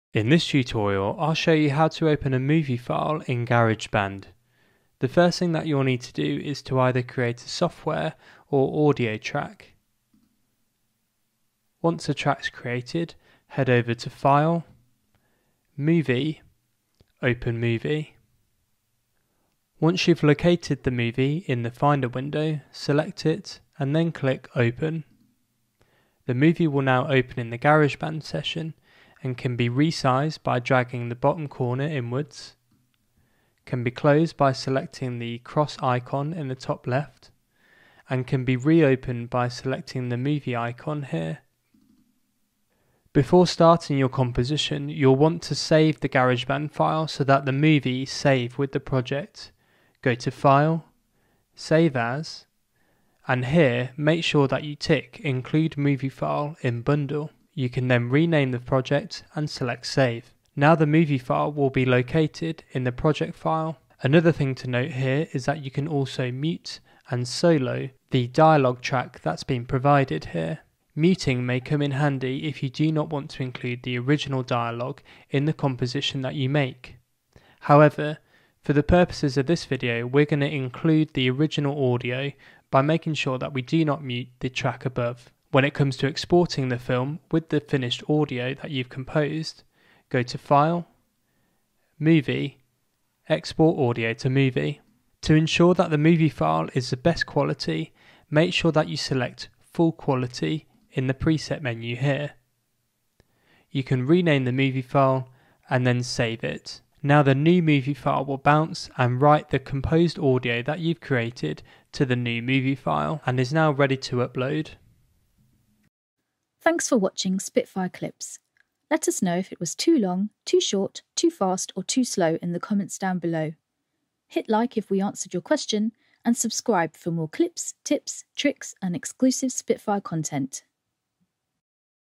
How to Import and Export a Video File into Garageband
How to open a video clip in Garageband.

0:00 Intro
0:05 How to Import a Movie File into Garageband
0:26 How to resize and close a Movie File in Garageband
0:45 How to save a Garageband Project with the Movie attached
1:05 How to Mute and Solo the Original Movie Track
1:25 How to Export a Movie File out of Garageband
1:56 End